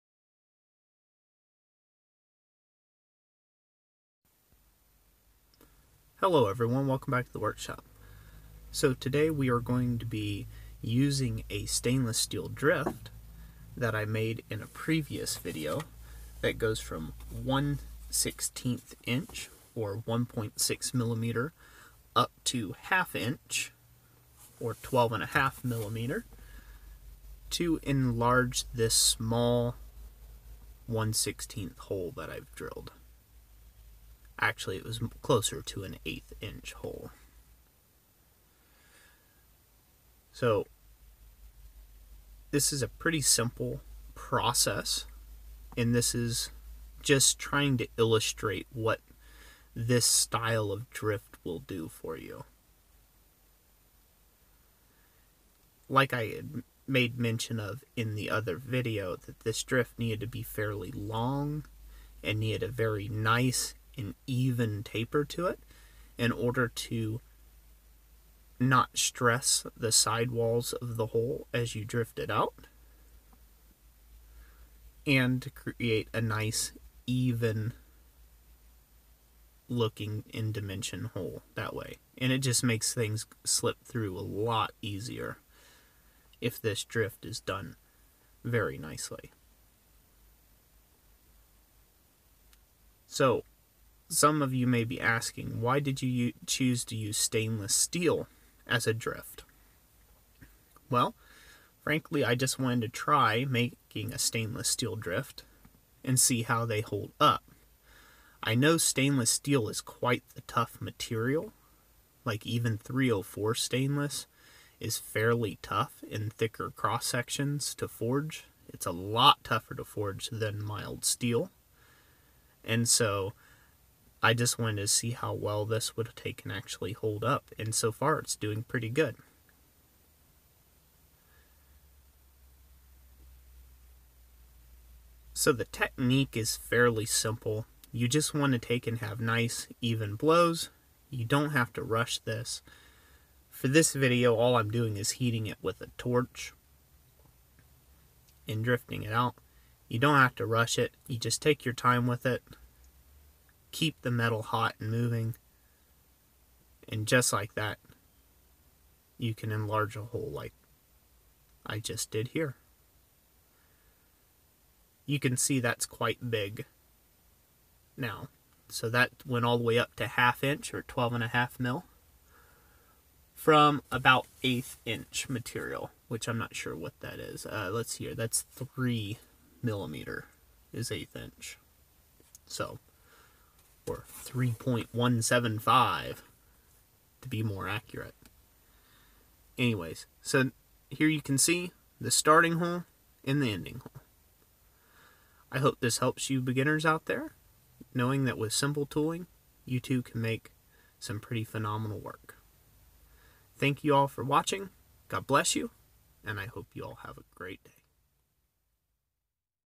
Tips on Using a Blacksmith Drift
In this video, I share my TOP TIPS for using a blacksmith drift. I show you how you can use a blacksmith drift tool to drift the size hole you need in your blacksmithing project. If you like learning how to make your own blacksmithing tools (such as this drift) check out my blacksmith channel Christ Centered Ironworks.